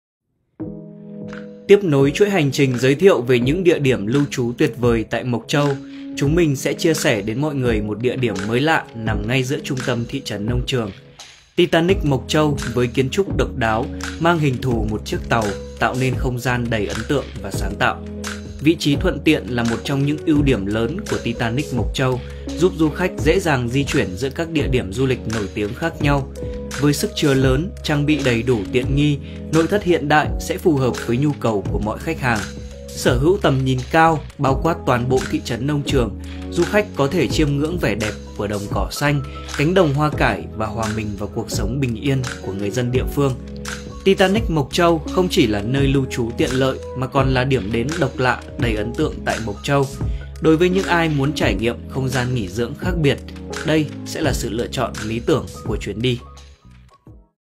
Titanic Moc Chau Homestay
Titanic Mộc Châu homestay là 1 homestay độc nhất vô nhị ở Tây Bắc với thiết kế hình 1 con thuyền lớn. Homestay Mộc Châu độc đáo này có 15 phòng nghỉ

Homestay Titanic Mộc Châu chia làm 3 khu nghỉ, 1 khu trung tâm là con tàu 3 tầng với 10 phòng ngủ, 2 khu còn lại có 4 phòng ngủ, tất cả đều mới, hiện đại, trang bị tiện nghi đầy đủ.

Nằm ở trung tâm thị trấn Nông trường, homestay dễ tìm, dễ đến và tiện nghỉ ngơi, thăm quan các điểm du lịch tại Mộc Châu.

Titanic Mộc Châu homestay  cũng phục vụ các dịch vụ ăn uống cho khách có nhu cầu